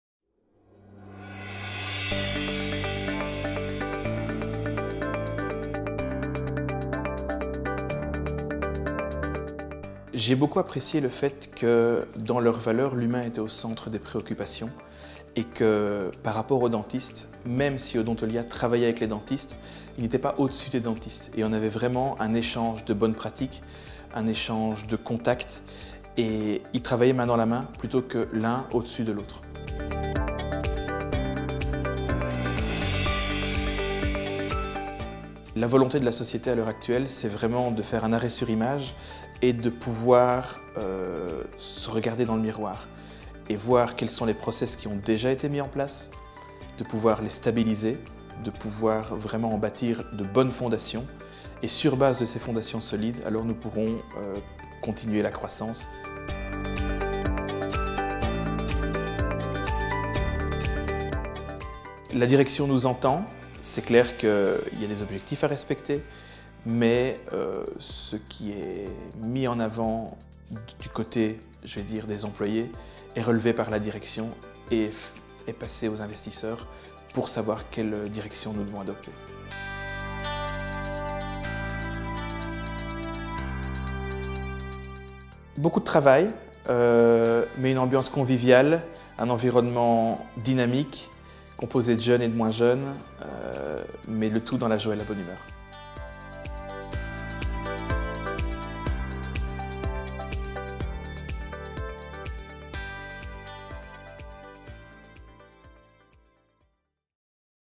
Benjamin Dieudonné - HR Project Manager
Benjamin Dieudonné legt uit waarom hij zich aangetrokken voelde tot de werking van Odontolia en de relatie van de medewerkers ten opzichte van het management in het kader van besluitvormingsprocessen.